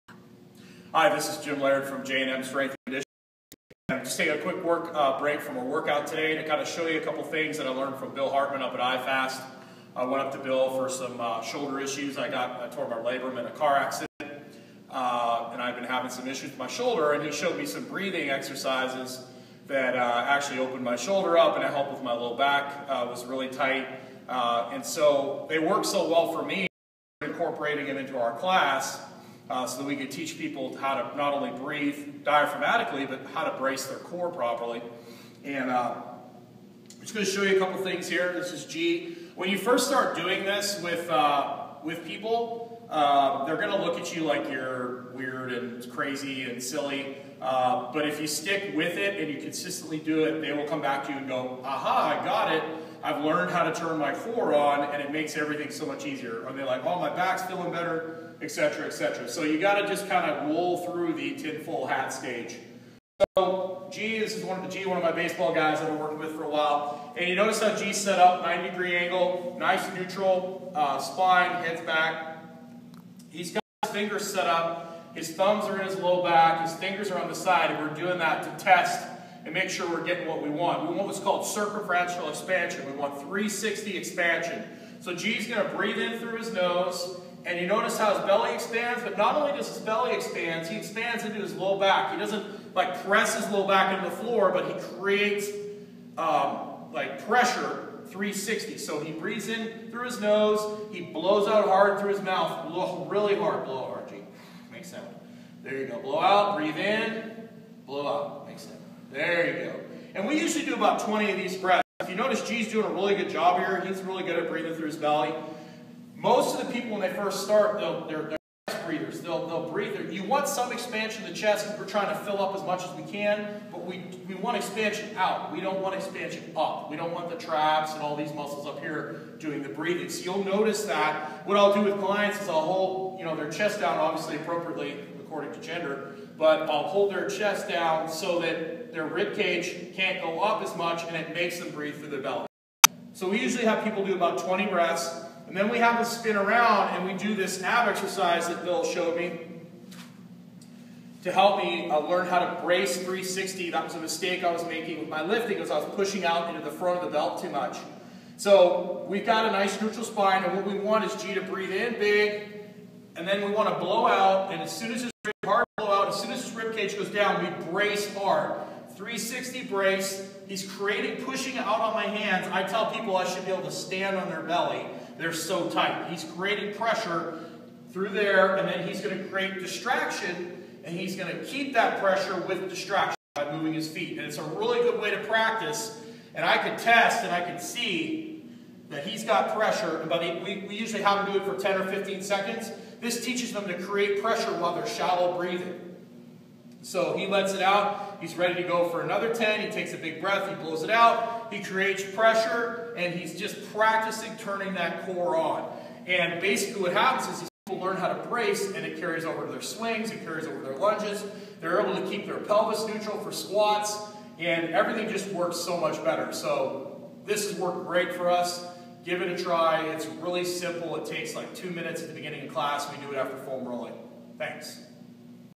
Breathing and bracing drills abdominal bracing powerlifting program Mike Robertson
Breathing and bracing drills we got from Bill Hartman at IFAST that we use in our classes at Gym Laird Strength and Conditioning.  I forgot to Q push into the wall on the second bracing drill.

mike Robertson
pri
zoa
abdominal bracing
powerlifting program